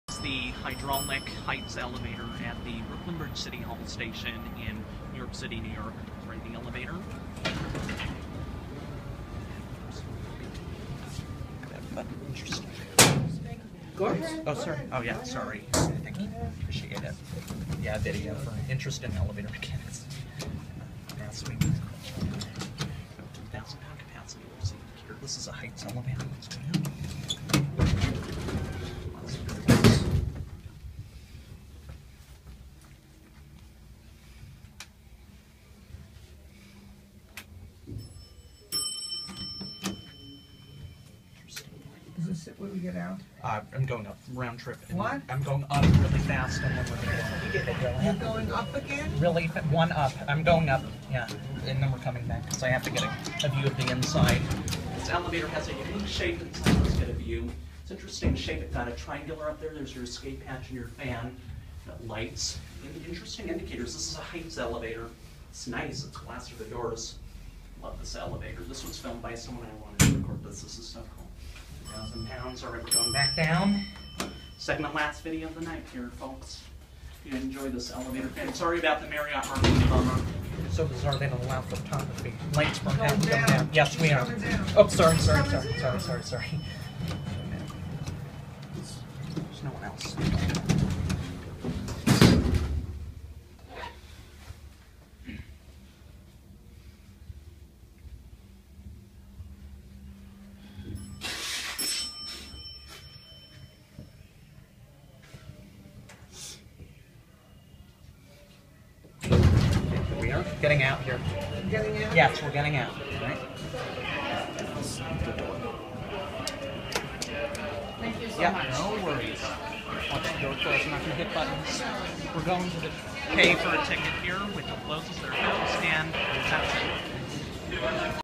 Scenic Heights Hydraulic Elevator @ Brooklyn Bridge-City Hall Station, NYC
This one is only scenic through the doors but is really cool. Cab interior has an interesting shape/layout. Installed in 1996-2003.